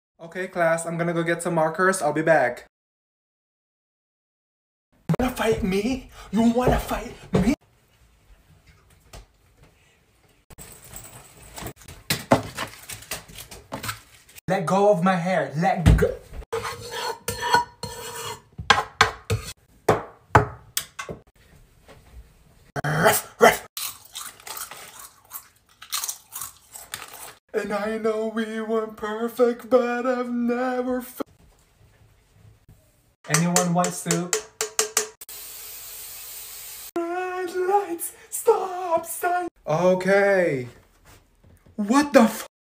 when the teacher leaves the class for 10 seconds
audio: lourdasprec

0:00 where it starts
0:03 when the teacher leaves
0:05 where the chaos starts
0:40 when the teacher returns